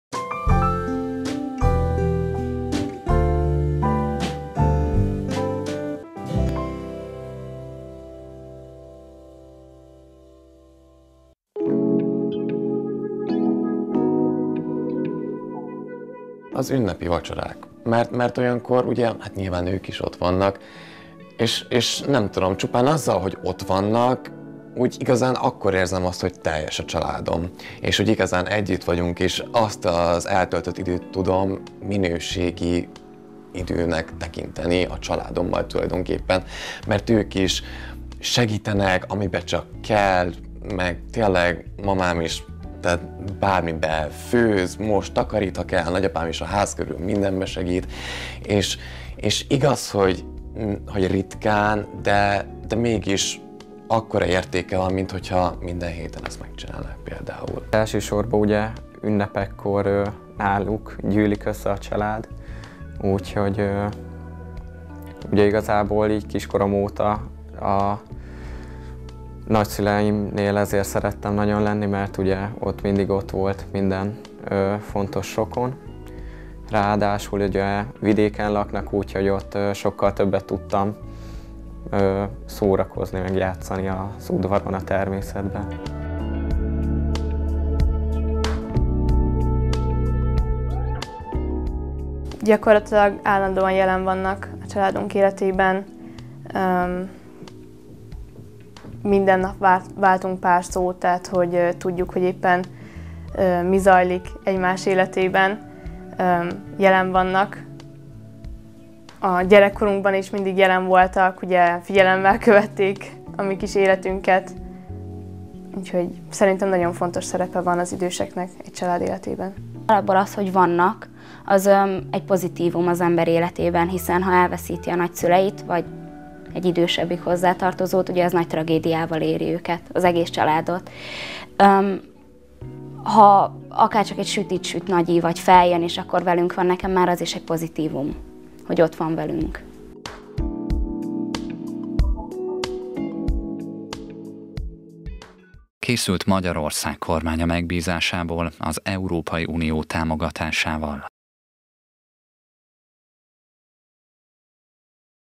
Útravalóul
Barátság, szerelem, család, tisztelet. Mindenkinek eszébe jut valami ezekről a szavakról: egy emlék, egy érzés, egy csalódás. Persze másképp gondolkodnak ezekről fiatalok és idősek. Az Útravalóul-ban arra próbáltunk választ kapni, mennyire látják hasonlóan vagy másként a korosztályok a különböző élethelyzeteket, és hogy milyen tanácsok fogalmazódnak meg az emberekben, ha szembe jönnek ezek a kifejezések.